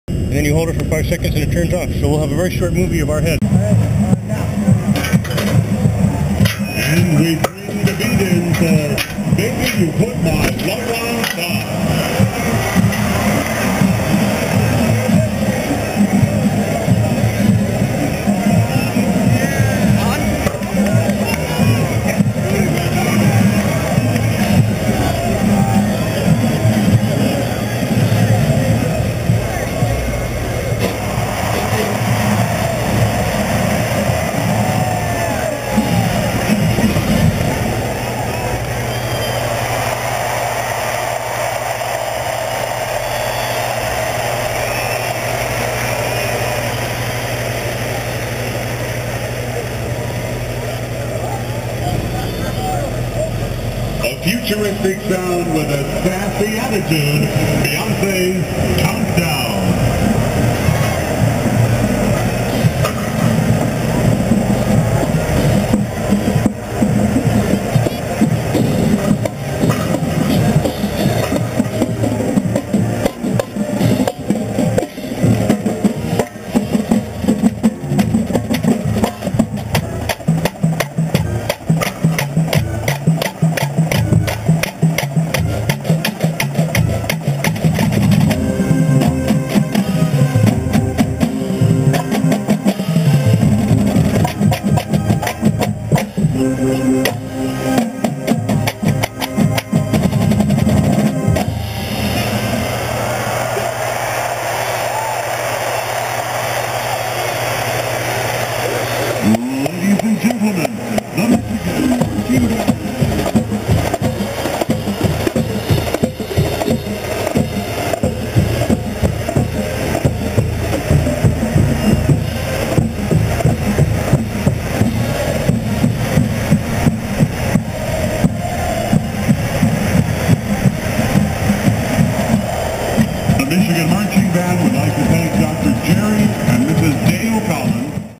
Notre Dame Michigan Halftime Mirror Ball GoPro
From the GoPro attached to the frame supporting the mirror ball.  This is an edited clip with highlights from when they drive out to just after the finale, as they are leaving.  You can see the GoPro change exposure as all the Synchrotron lights hit it.  (thanks to Jake & Krunal!)